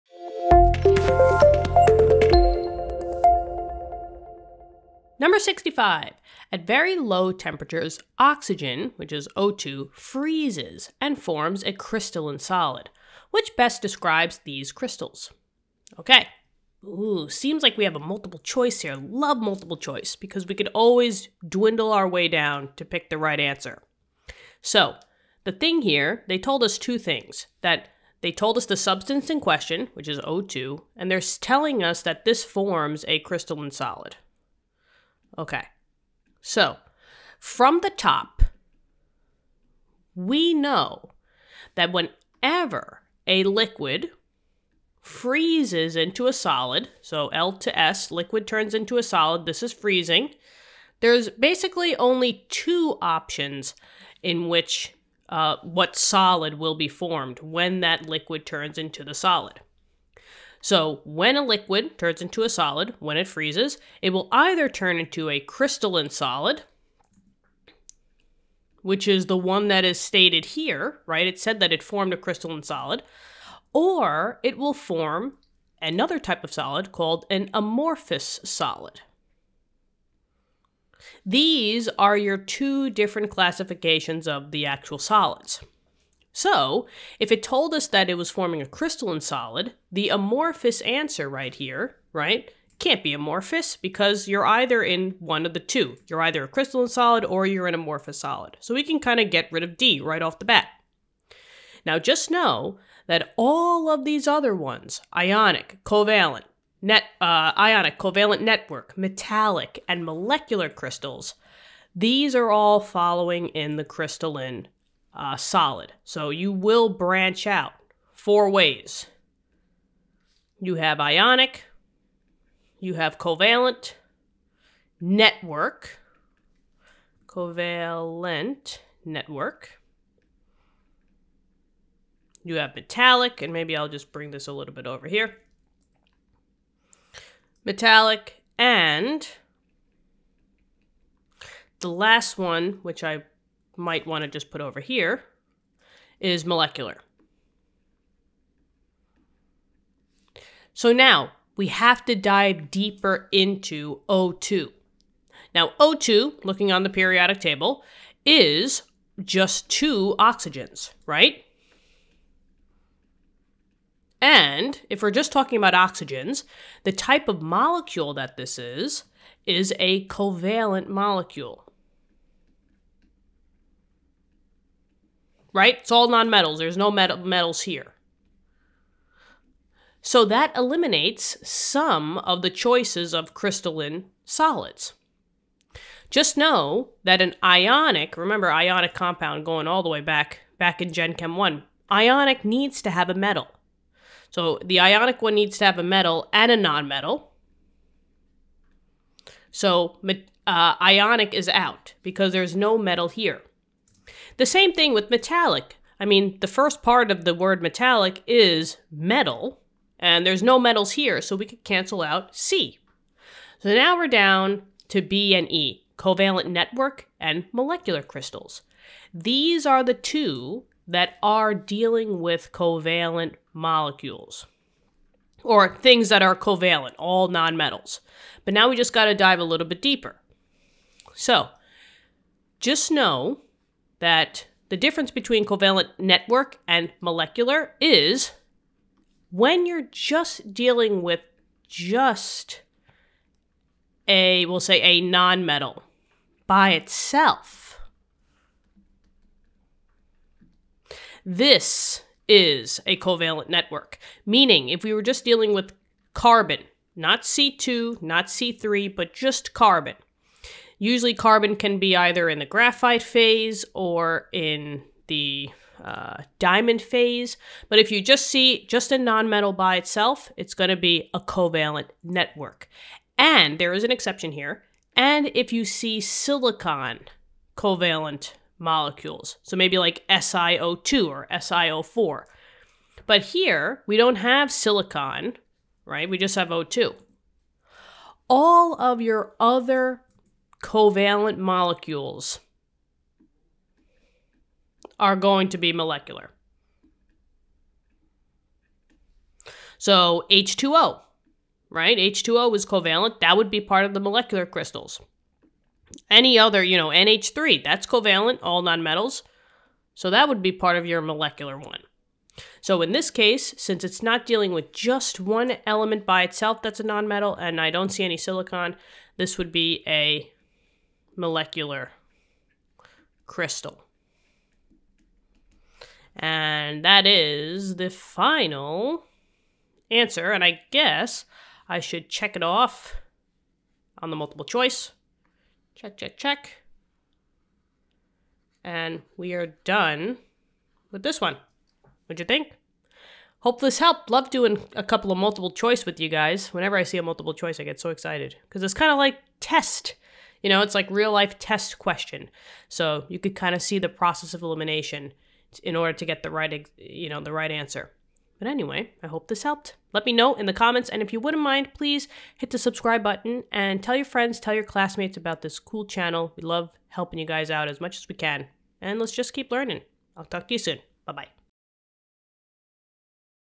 10.65 | At very low temperatures oxygen, O2, freezes and forms a crystalline solid. Which best
At very low temperatures oxygen, O2, freezes and forms a crystalline solid. Which best describes these crystals?
(a) ionic
(d) amorphous
(e) molecular crystals